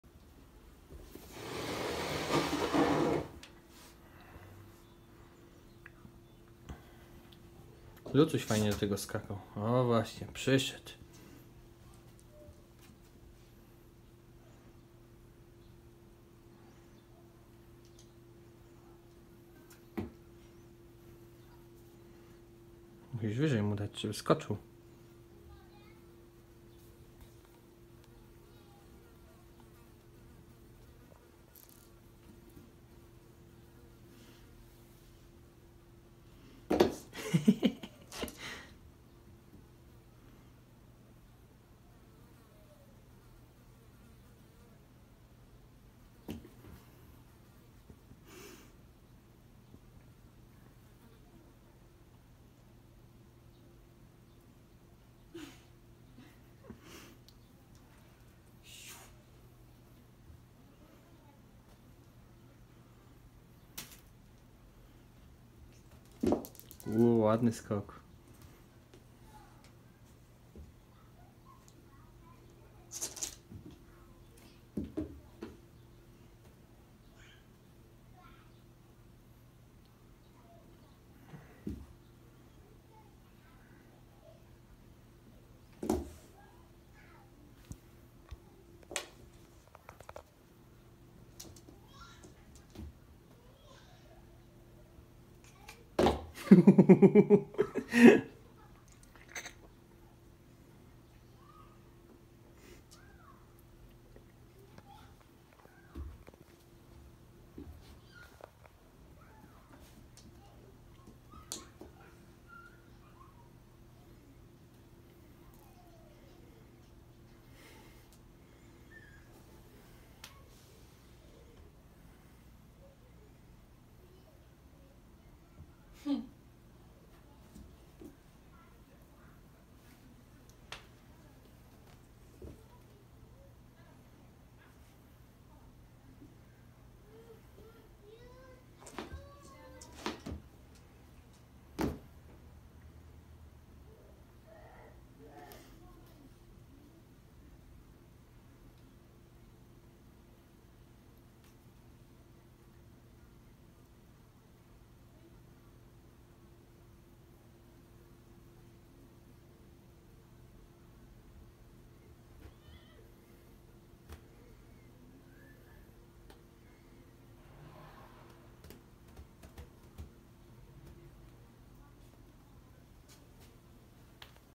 Zabawa z Kitku - Nowa Zabawka *wężyk na sznurku*
Kotki uwielbiają nowe zabawki, szczególnie te na sznurku ;P